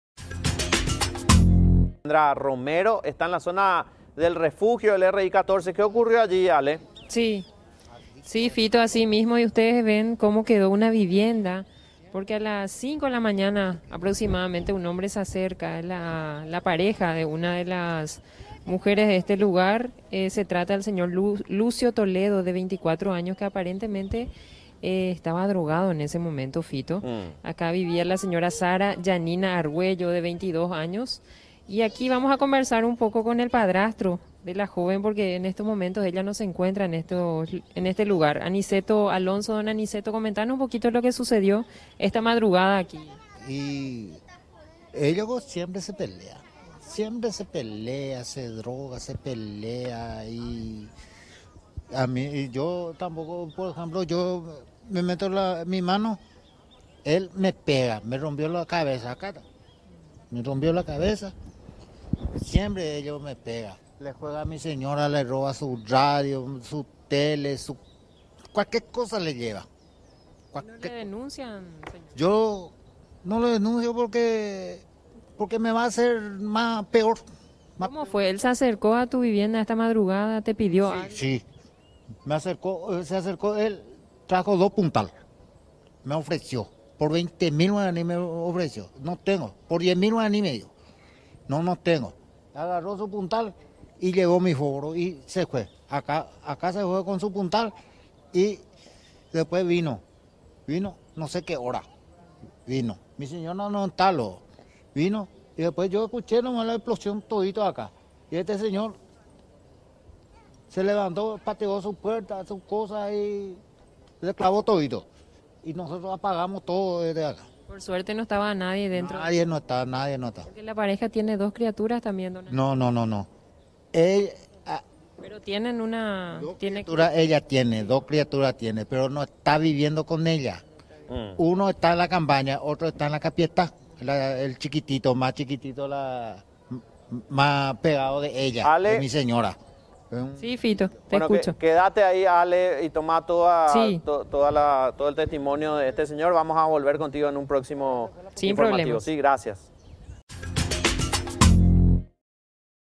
Quemó la casa de su ex novia por celos en refugio de damnificados
Un hombre decidió quemar la casa de su ex novia presuntamente por celos. Según testigos, el sujeto estaba drogado cuando incendió la precaria vivienda edificada en un refugio de la SEN.